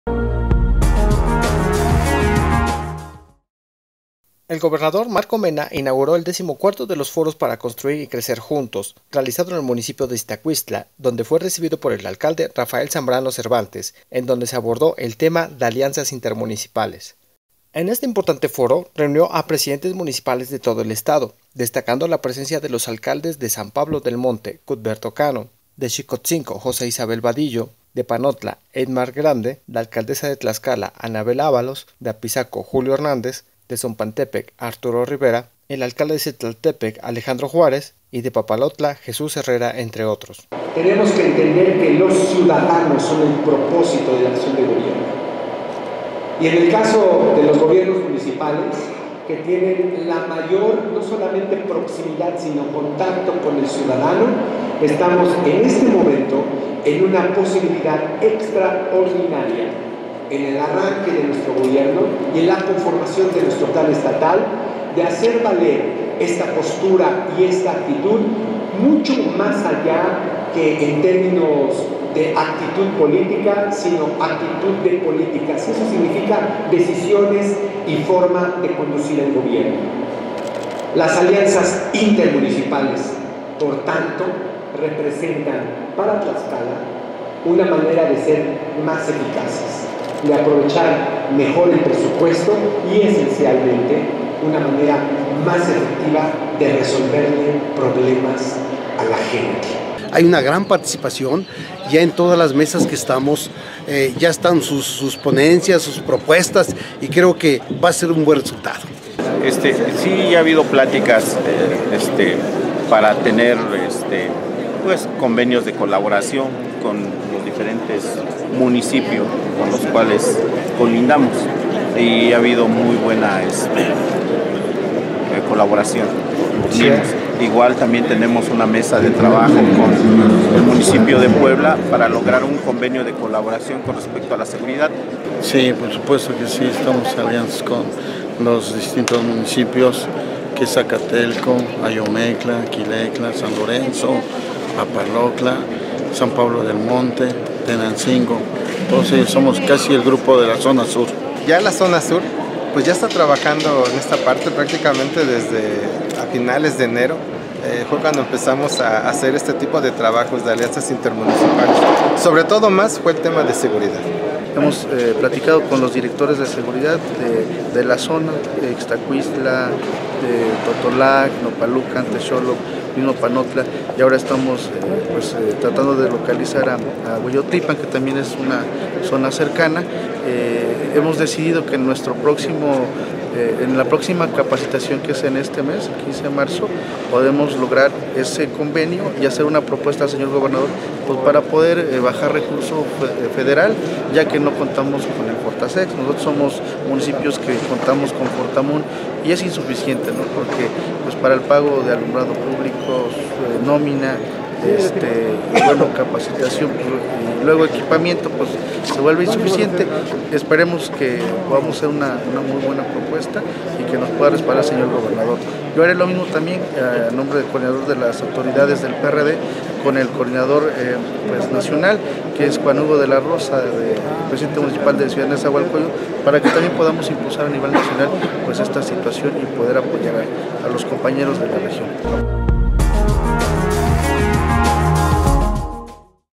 Alianzas Intermunicipales: siguiente nivel de éxito: MMR
El gobernador Marco Mena inauguró el decimo cuarto de los Foros para Construir y Crecer Juntos, realizado en el municipio de Ixtacuixtla, donde fue recibido por el alcalde Rafael Zambrano Cervantes, en donde se abordó el tema de “Alianzas Intermunicipales”.

 En este importante foro reunió a presidentes municipales de todo el estado, destacando la presencia de los alcaldes de San Pablo del Monte Cutberto Cano,  Xicohtzinco José Isabel Badillo, de Panotla Eymard Grande, de Apizaco, Julio Hernández, la alcaldesa de Tlaxcala, Anabell Ávalos, de Tzompantepec Arturo Rivera…
Alejandro Juárez alcalde de Zitlaltepec
 y de Papalotla Jesús Herrera entre otros.